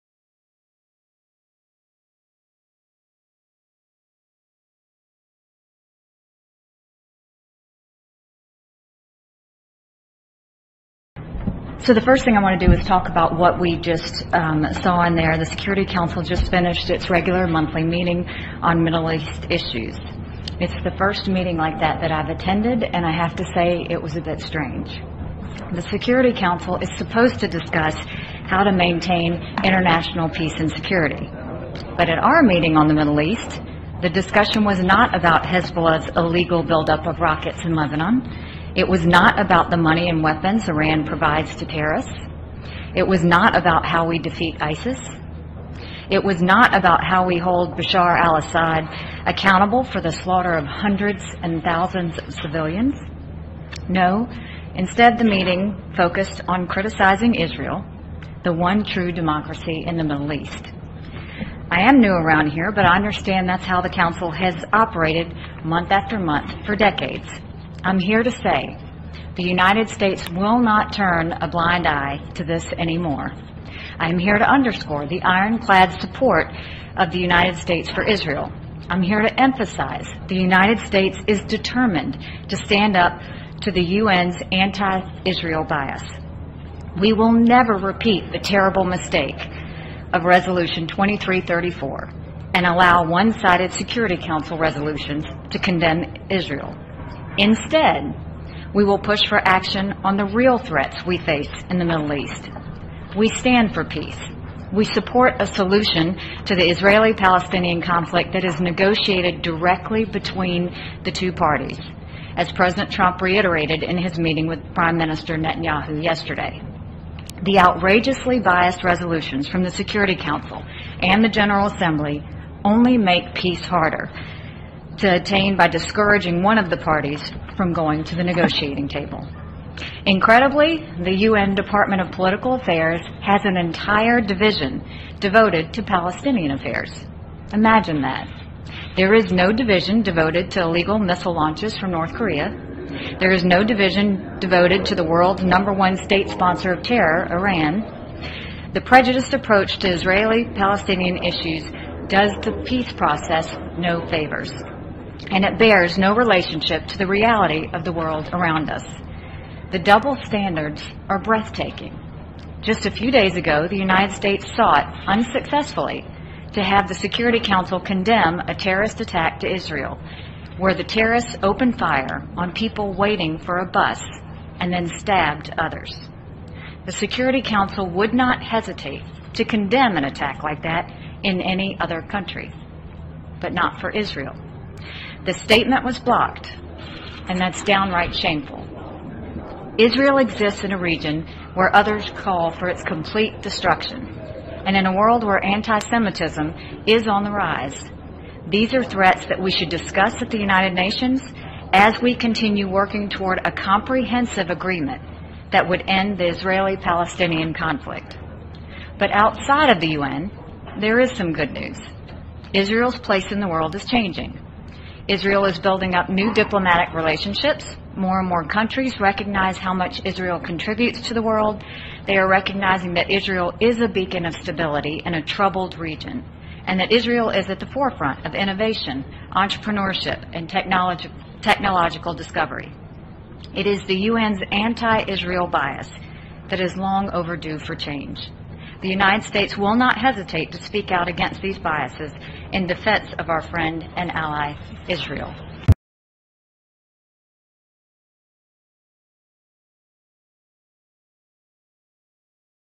Nikki Haley Lambastes the UN Over its Treatment of Isreal
Trumped appointed Haley as US Ambassador to the UN, in spite of any negative comments she had previously made about him.  This is a sign of a leader who makes objective decisions, rather than emotional ones.  His choice appears to be paying off.